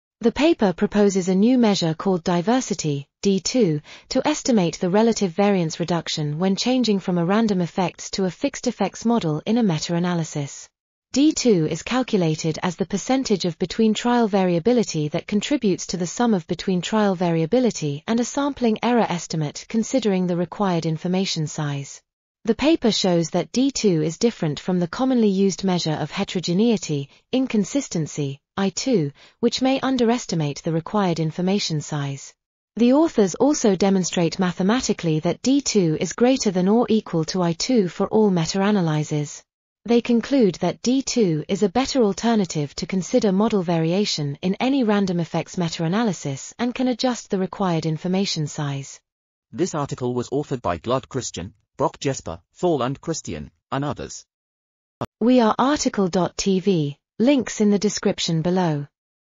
Estimating required information size by quantifying diversity in random-effects model... | RTCL.TV
### Keywords ###

### Article Attribution ###
Title: Estimating required information size by quantifying diversity in random-effects model meta-analyses
Authors: Gluud Christian, Brok Jesper, Thorlund Kristian ,and Wetterslev Jørn
Publisher: BMC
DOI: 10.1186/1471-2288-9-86

### Video Timestamps ###
0:00:00 - Summary
0:00:49 - Title
0:00:55 - Tail
0:00:59 - End